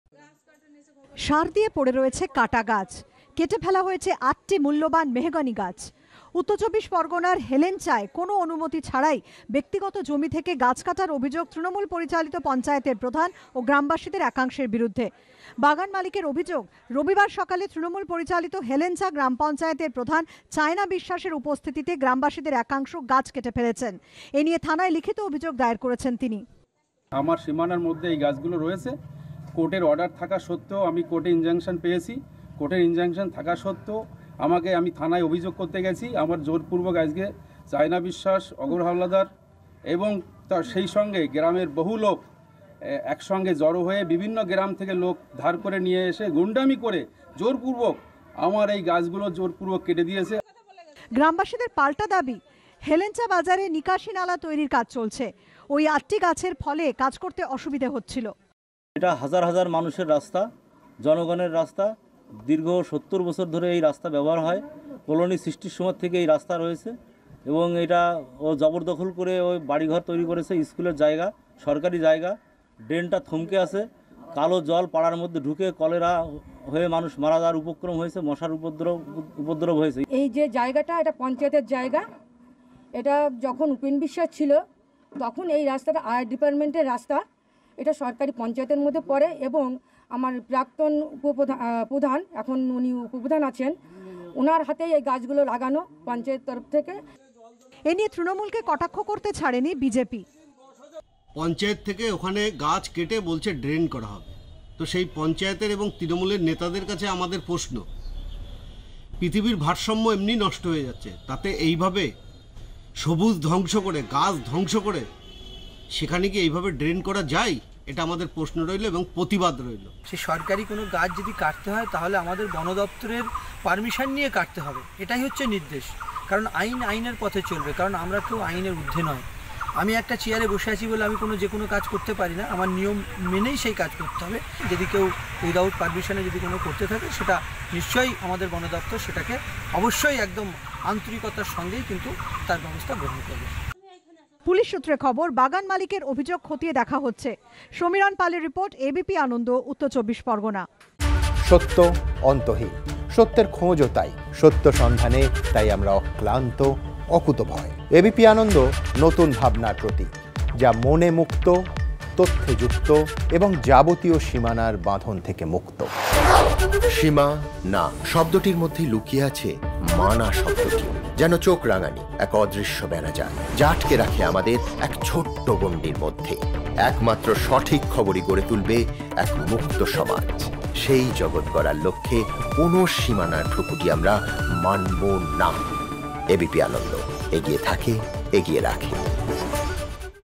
Bagda: ব্যক্তিগত মালিকানার গাছ কাটায় অভিযুক্ত পঞ্চায়েত প্রধান, দিচ্ছেন জনরোষের তত্ত্ব।Bangla News
বাগদার হেলেঞ্চাতে ব্যক্তিগত মালিকানার ৮টি গাছ কেটে দেওয়ার অভিযোগ উঠল প্রধান ও গ্রামবাসীদের বিরুদ্ধে। জনরোষের জেরে গাছ কাটা হয়েছে, সাফাই দিয়েছেন পঞ্চায়েত প্রধান। বিষয়টি খতিয়ে দেখা হচ্ছে বলে জানিয়েছেন জেলা পরিষদের বন কর্মাধ্যক্ষ।